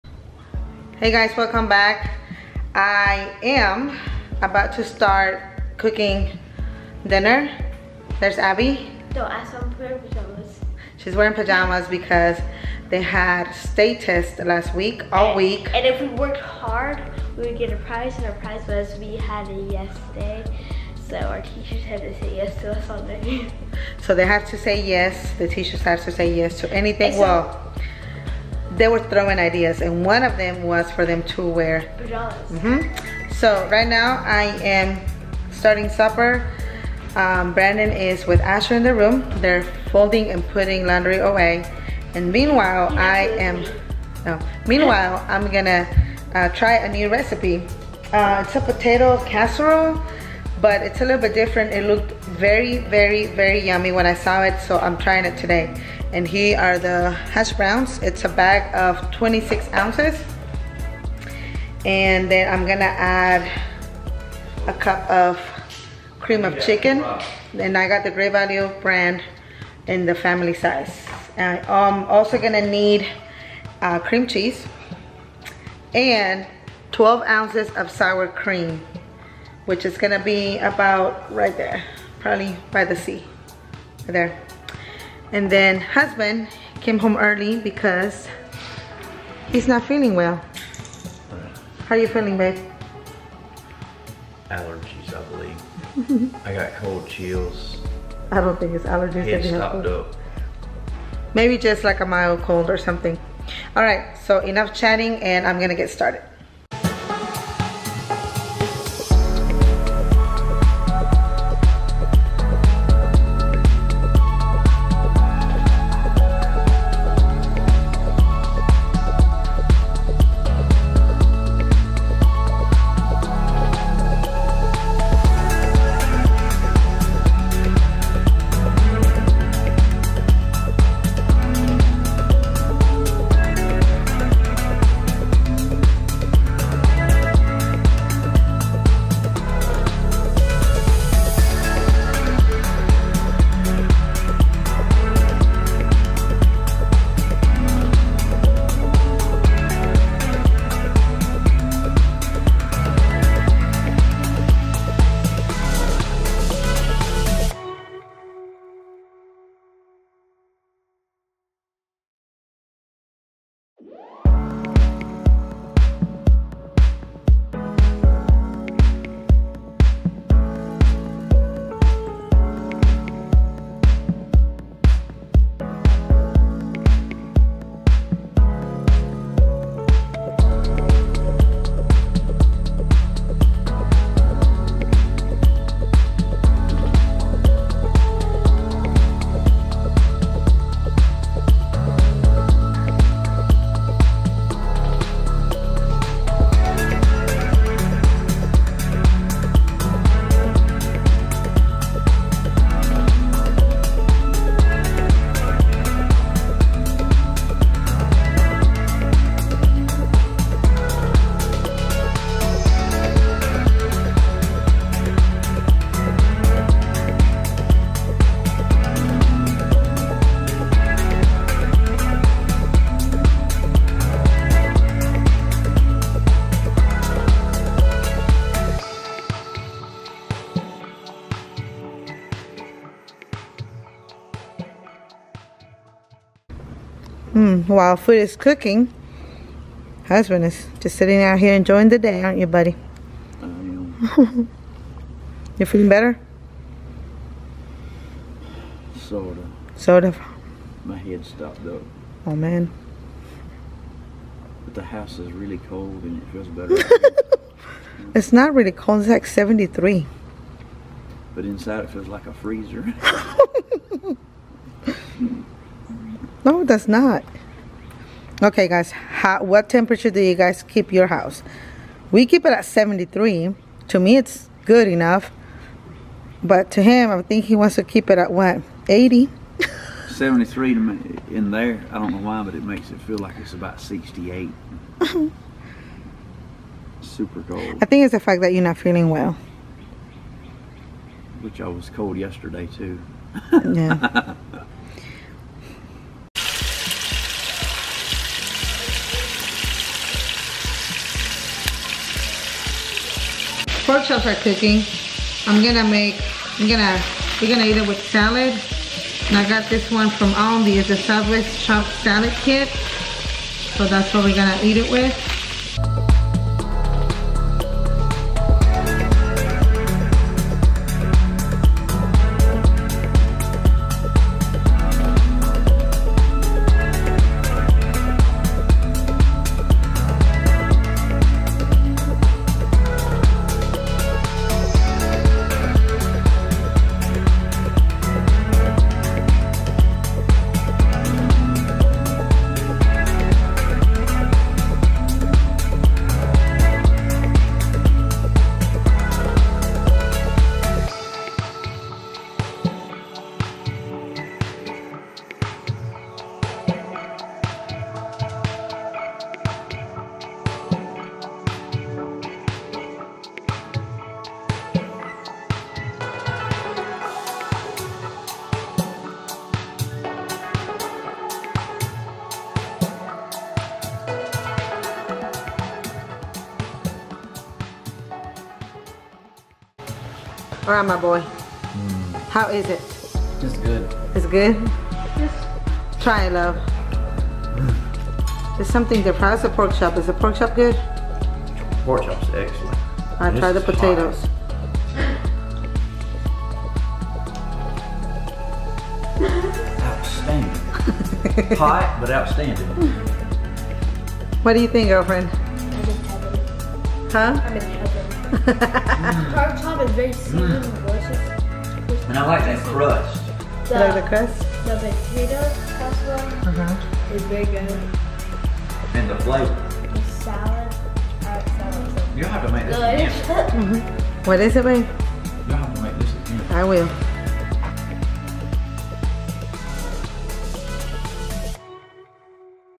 DITL- COOKING A NEW RECIPE FAMILY APPROVES
FUNERAL POTATOES
2 24OZ BAGS OF HASH BROWNS
1 LARGE CAN CREAM OF CHICKEN  (GREAT VALUE )
12 OZ OF SOUR CREAM
ABOUT A QUARTER OF  WHITE ONIONS
SEASONED WITH ONION POWDER
BLACK PEPPER
I ADDED SALT
I ALSO ADDED 8OZ OF CREAM CHEESE (OPTIONAL)
8OZ OF SHARP CHEDDAR CHEESE
4OZ OF MILD CHEDDAR
CRUSH 2 PACKS OF RITZ CRACKERS ADD 1/2 STICK OF BUTTER
COVER WITH ALUMINUM FOIL

REMOVE ALUMINUM FOIL CONTINUE BAKING FOR 20 MORE MITUES.